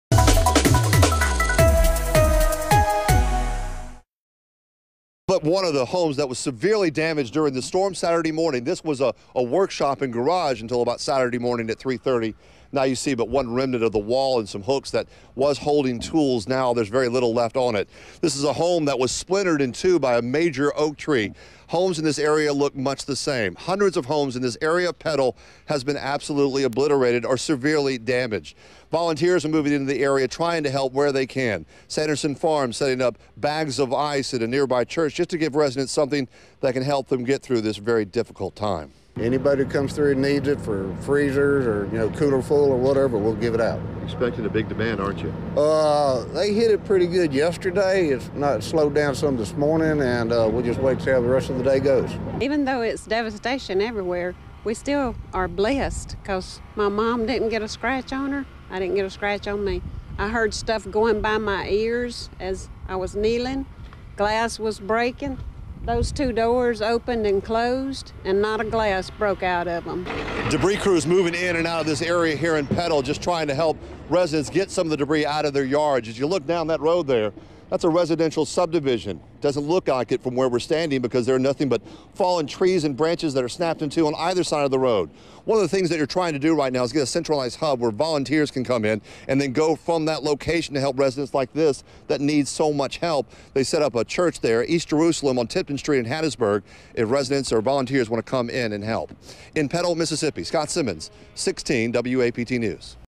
Petal hit hard by tornado
Tornados killed four people and damaged almost one thousand homes ih the Hattiesburg area.